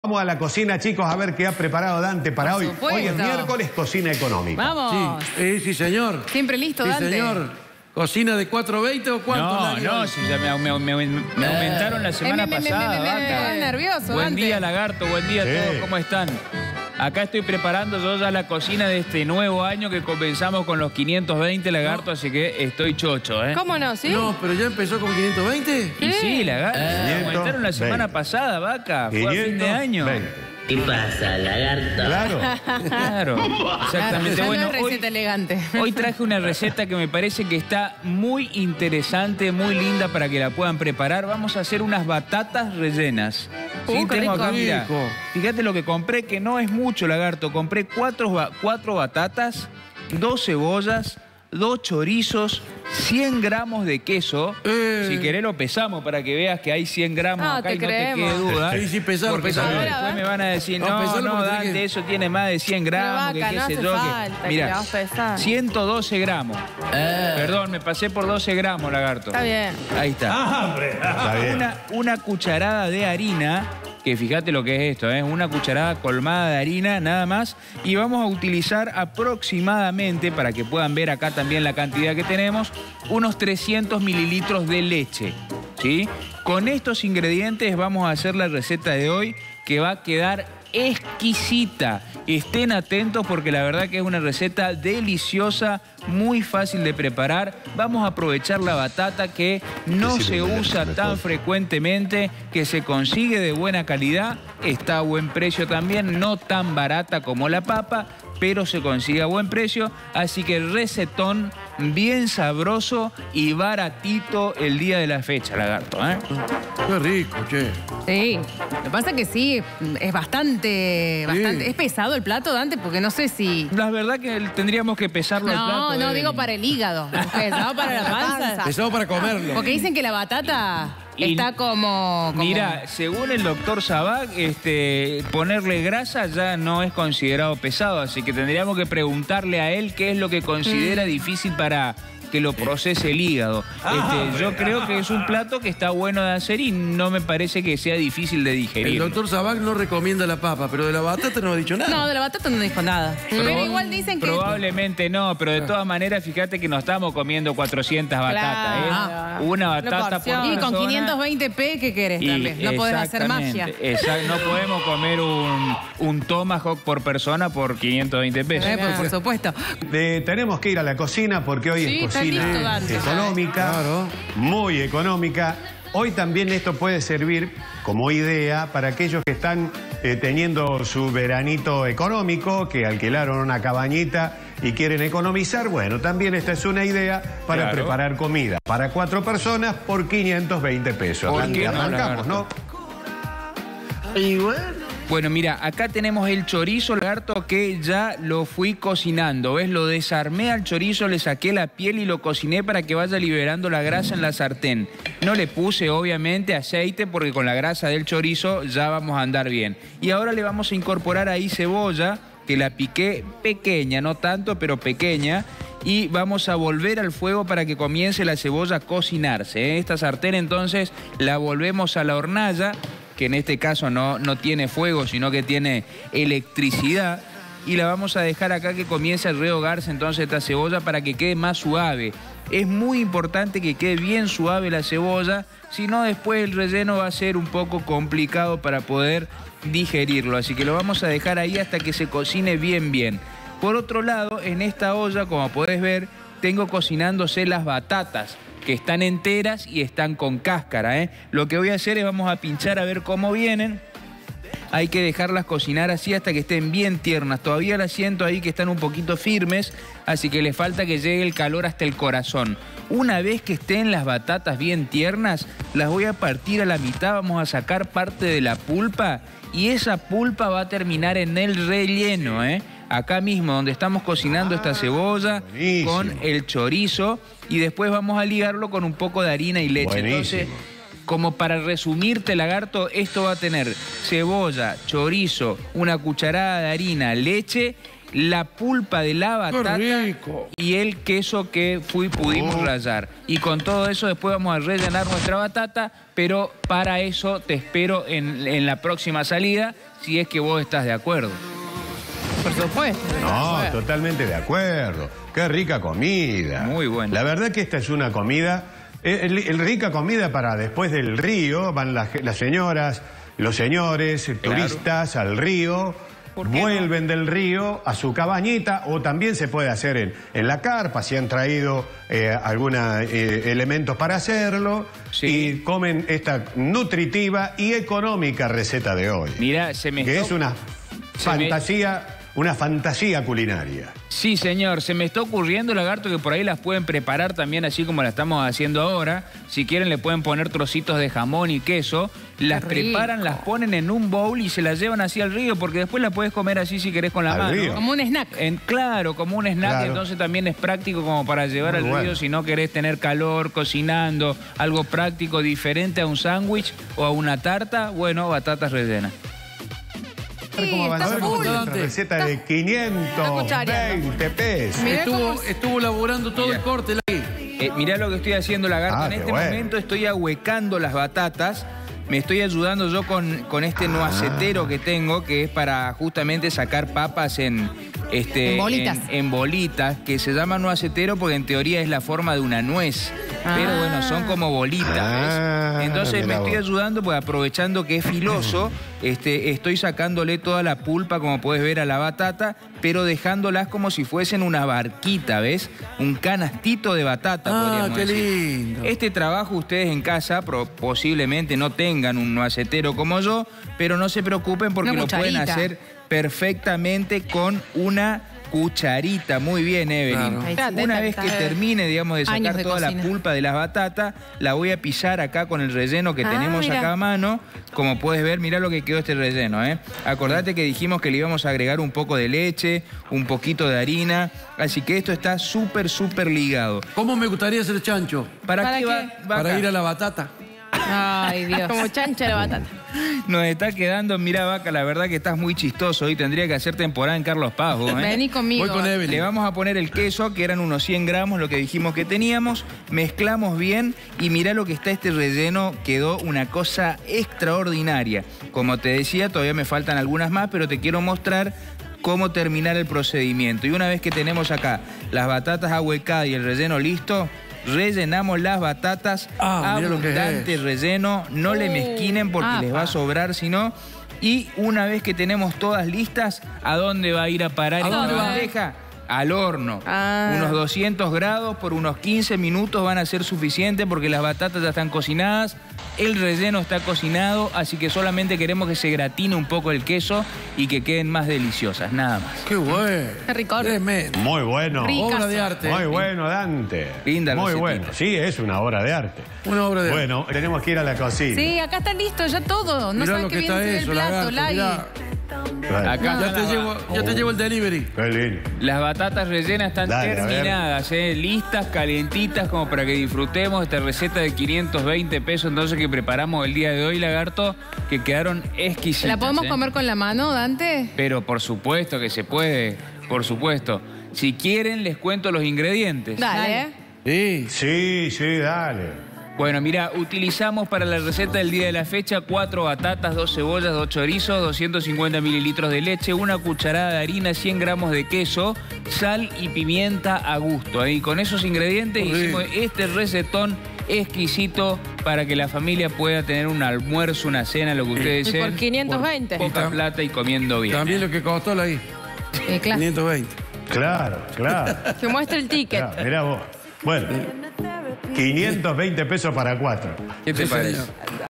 Batatas rellenas receta de Dante Enriquez
Hoy Dante Enriquez prepara batatas rellenas